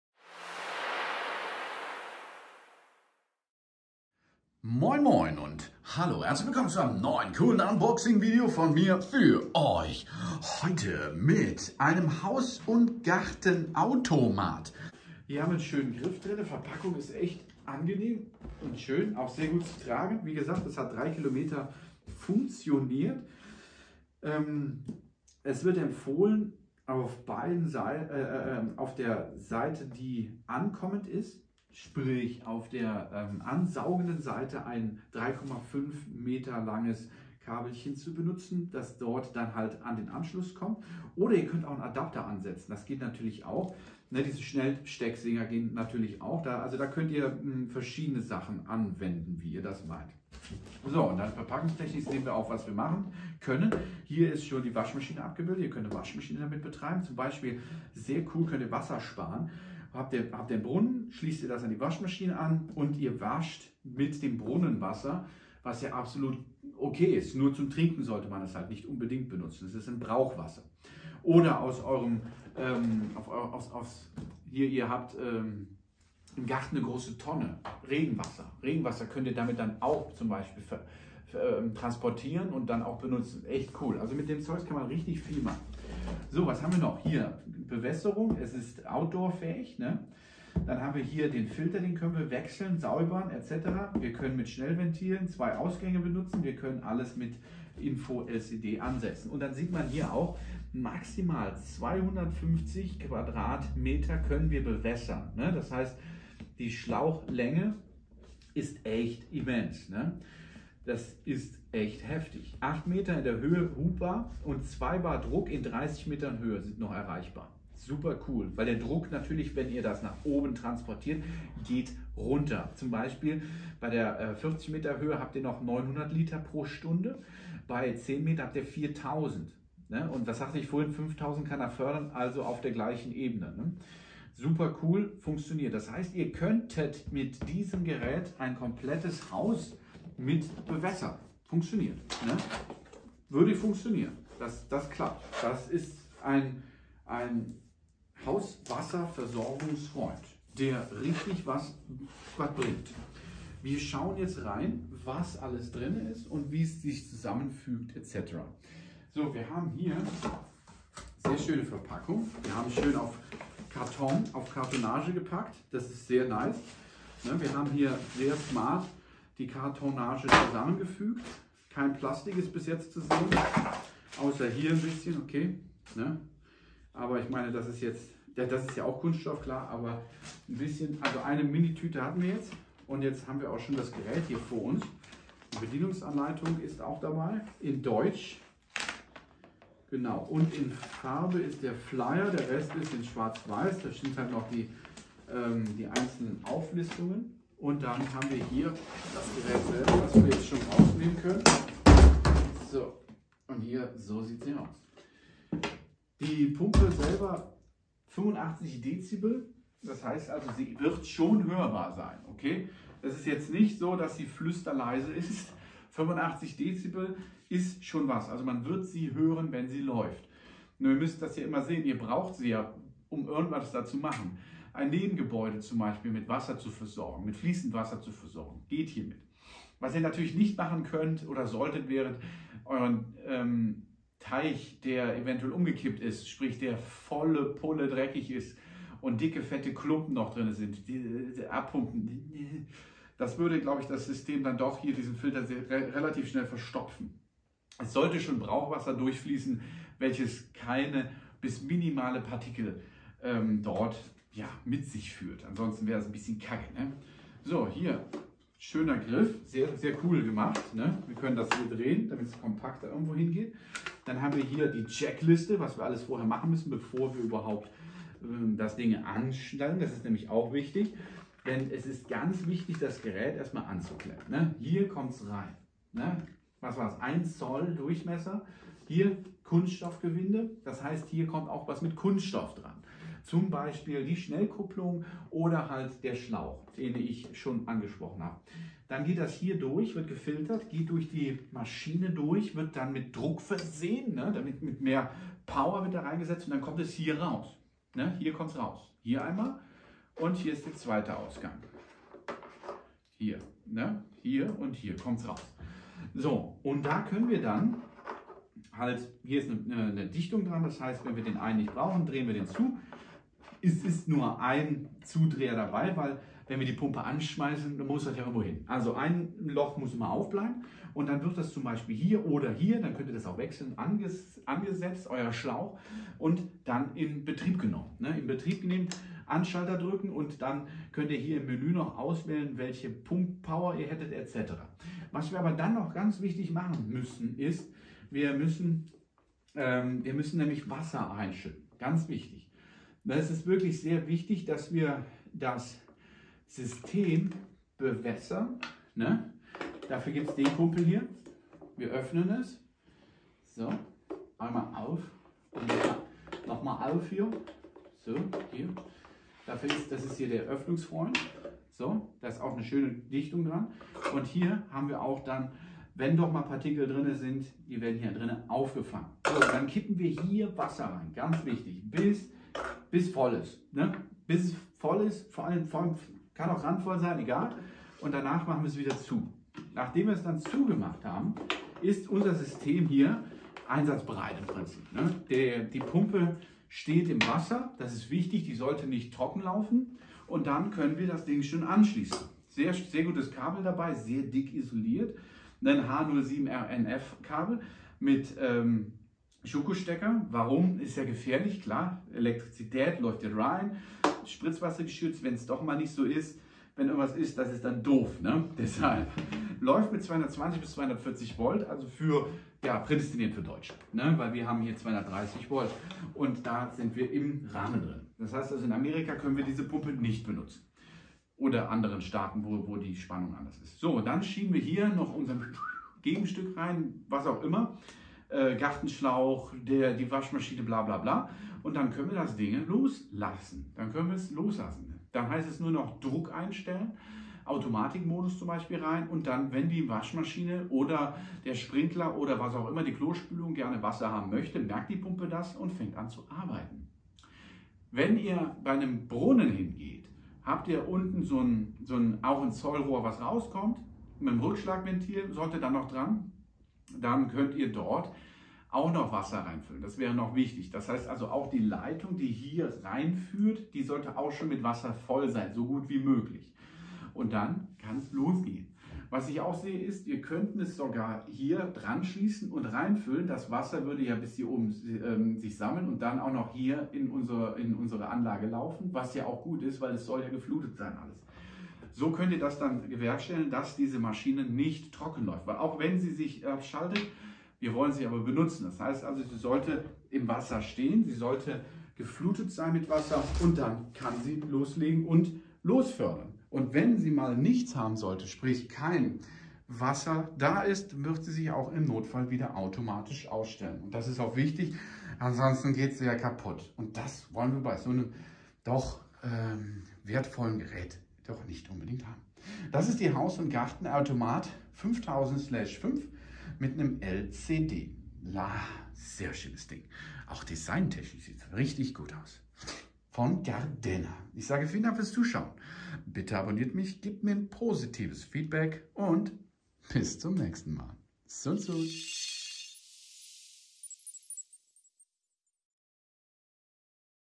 Gardena Haus- & Gartenautomat 5000/5 LCD für Garten, Brunnen, WC & WaMa - Profi Brunnen Unboxing
Der kompakte GARDENA Haus- & Gartenautomat 4000/5 mit einer Nennleistung von 300 Watt, einer maximalen Fördermenge von 5.000 l/h und einem max. Druck von 5,0 bar eignet sich hervorragend für den mobilen und stationären Einsatz, egal wo man ihn benötigt. Mit dem Hauswasserautomaten spart man Geld und schont die Umwelt, indem man kostenloses Regen- oder Brunnenwasser zur Bewässerung des Gartens oder im Haus für die Toilette oder für die Waschmaschine verwendet. Zwei Ausgänge, einer davon schwenkbar, ermöglichen den gleichzeitige Betrieb von zwei Anschlussgeräten. Die integrierte Elektronik aktiviert den robusten Hauswasserautomaten vollautomatisch bei Wasserbedarf und schaltet ihn danach wieder aus. Die Bedienung ist einfach, störungsfrei und komfortabel. Man kann den Hauswasserautomaten dank des ergonomischen Tragegriffs leicht transportieren, per Knopfdruck in Betrieb nehmen und durch einfache Bedienung die zahlreichen Funktionen nutzen. Durch die neuartige integrierte Rückschlagventil-Technik ist ein schnelles Ansaugen möglich. Das neuartige, eingebaute Kleinmengen-Programm sichert auch bei geringen Fördermengen einen problemlosen Betrieb, z. B. bei modernen Waschmaschinen oder Tropfbewässerungsanlagen. Die Pumpe verfügt darüber hinaus über ein Fehlerdiagnosesystem. So werden kleinste Störungen, beispielsweise ausgelöst durch tropfende Wasserhähne, undichte Spülkästen oder saugseitige Probleme, registriert. Ein großes LC-Display ermöglicht die Kontrolle aller Pumpenfunktionen, und gibt ausführliche Text-Informationen zu evtl. Störungen und deren Abhilfe. Eine Trockenlaufsicherung verhindert zudem die Beschädigung des Gerätes bei ausbleibendem Wasser. Ausschließlich hochwertige Materialien sind für einen leistungsstarken und geräuscharmen Betrieb in der Pumpe verbaut. So befördert ein wartungsfreier Kondensatormotor mit Thermoschutzschalter das Wasser durch ein Jet-Pump-System per Druck effizient an Ort und Stelle. Für die einfache Handhabung erleichtert eine groß.

pduesp